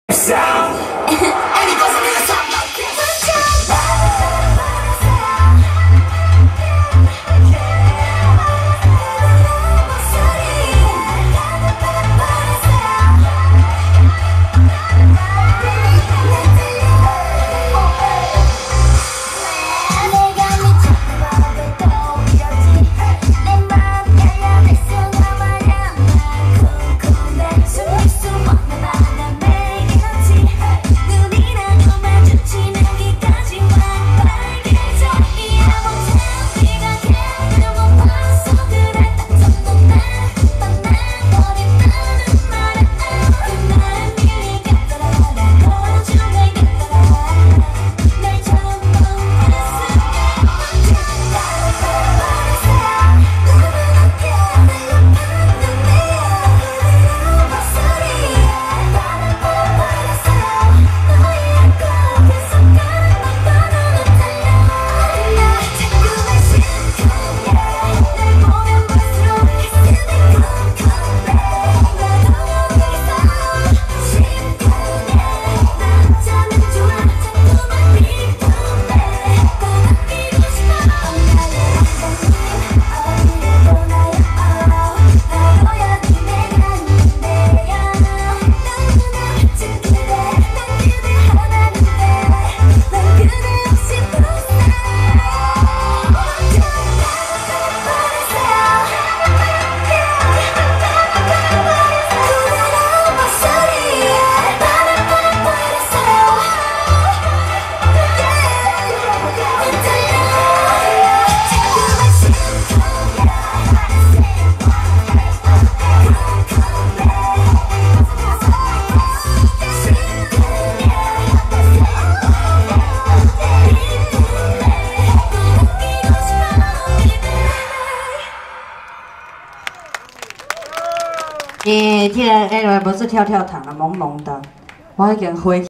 20190630 Kiyomi萌萌噠舞者表演/7-6，台南歸仁進興宮三載圓科進香回鑾遶境大典。
20190630 Kiyomi萌萌噠舞者表演/7-6，台南歸仁進興宮三載圓科進香回鑾遶境大典。